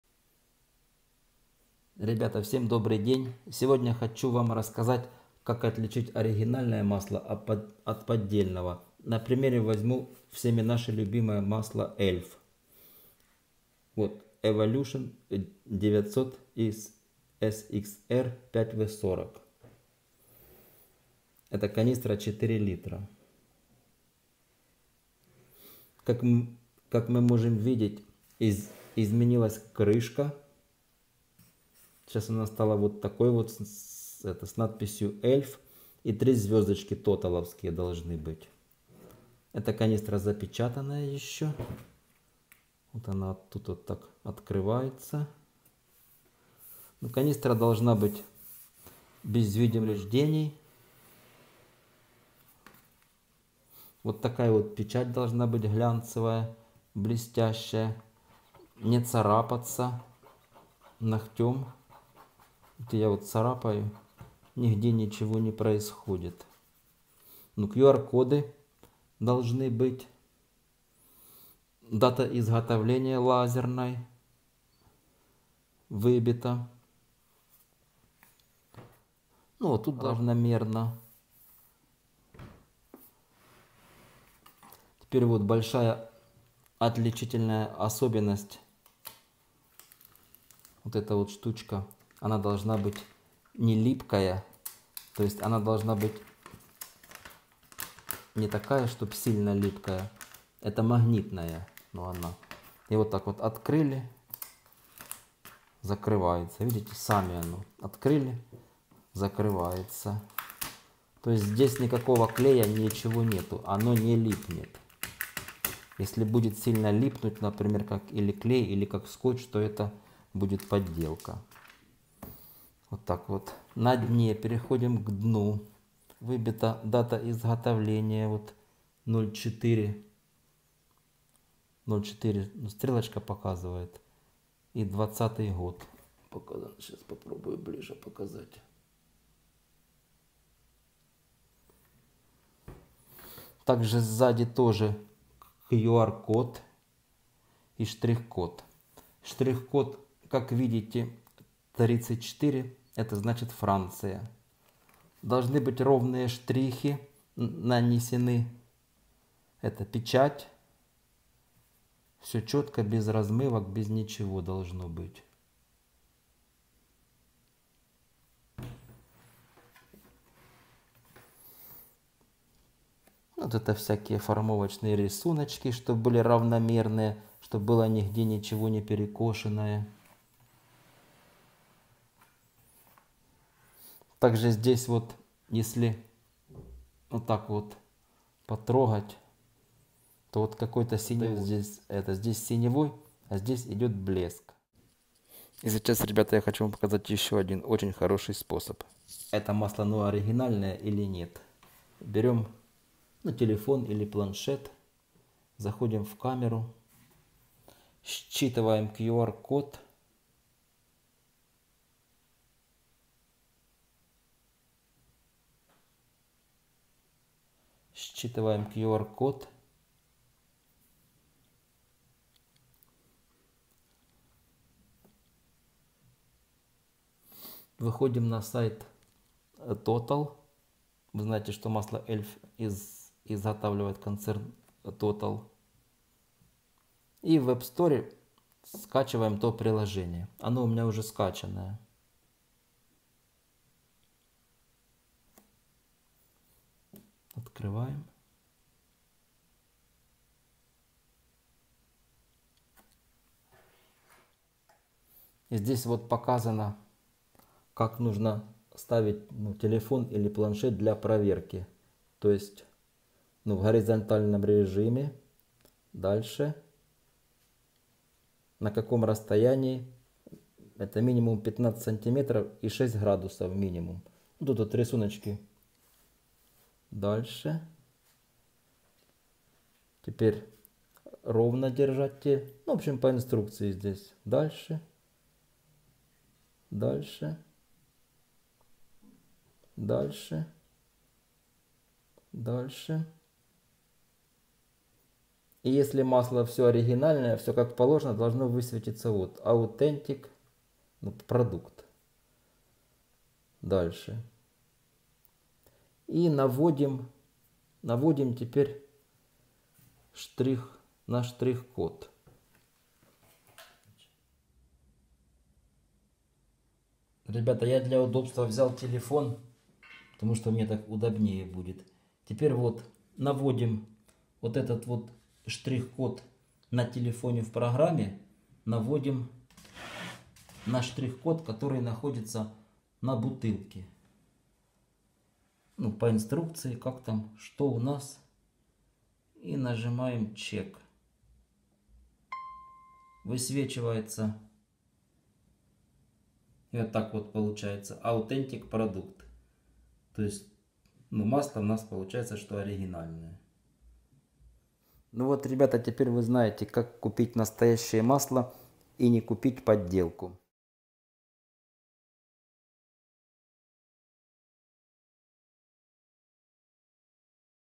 Моторное масло Elf. /как отличить подделку / масло ELF EVOLUTION 900 SXR 5W40
Сегодня решил  показать Вам как отличить оригинальное моторное масло Эльф/ELF/  от подделки. А самое главное какое приложение в Вашем смартфоне сможет это сделать. Масло Эльф Эволюшн 5в40. ELF EVOLUTION 900 SXR 5W40. Приложение Total#ELF#эльф#маслоЭльф#ELFEVOLUTION900SXR4W40#